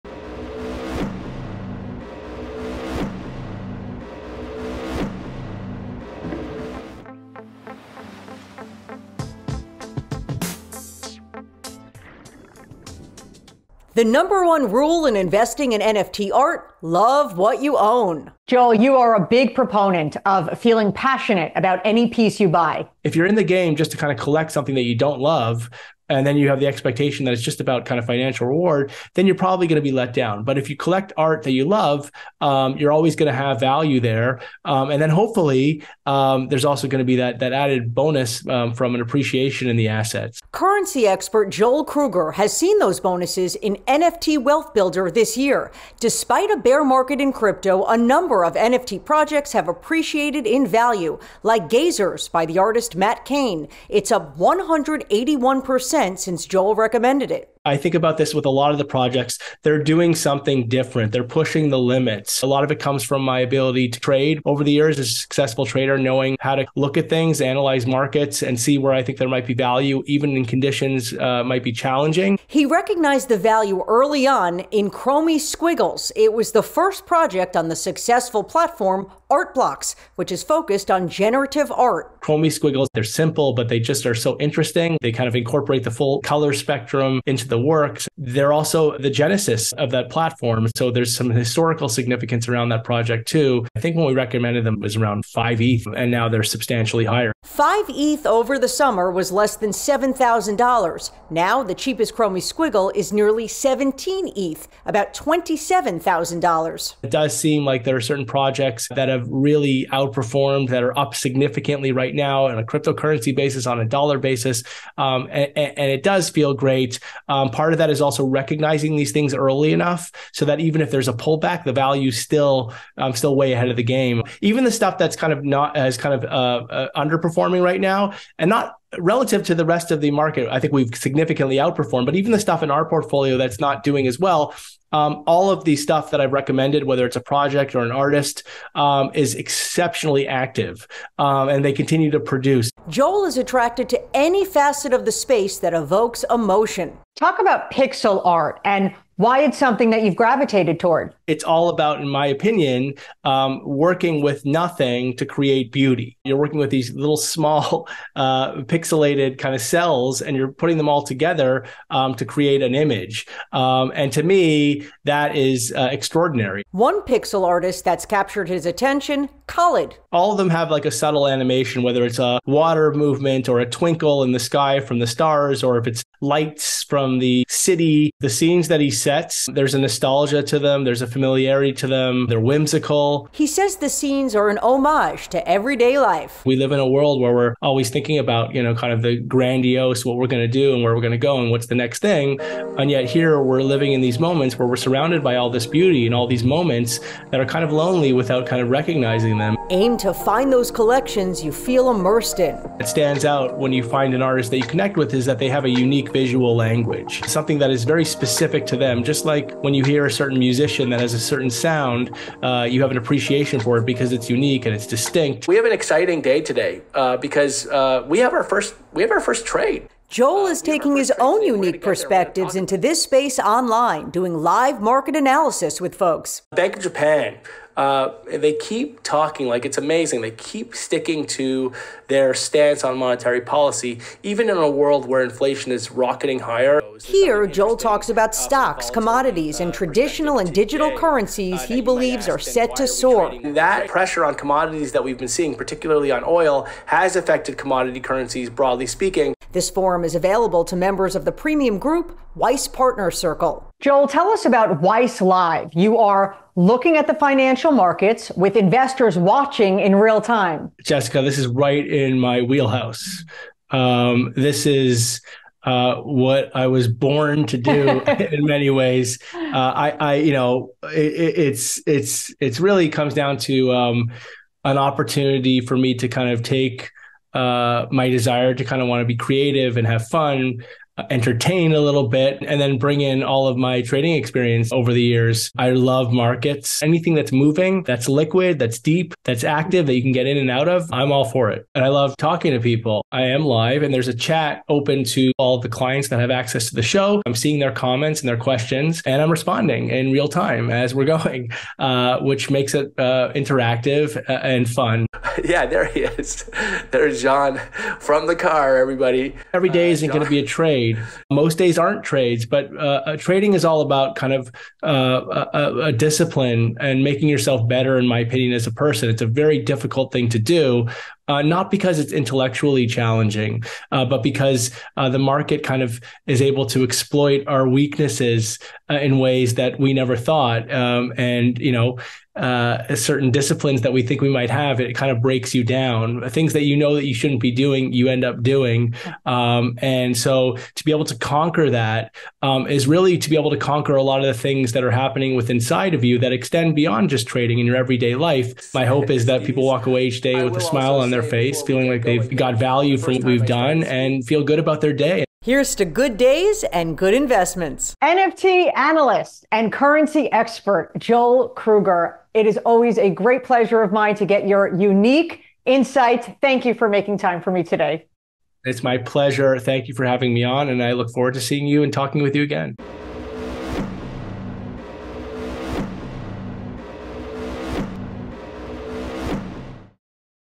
NFT Art Is Breaking New Ground
Despite a bear market in crypto this year, facets of the NFT art sector are skyrocketing in value.

In this segment, you’ll see vibrant digital artwork, as NFT Analyst Joel Kruger and Financial News Anchor Jessica Borg discuss what makes their “their distinct visual language” both appealing and potentially hugely profitable.  They also explore growing platforms that are shaking up the space.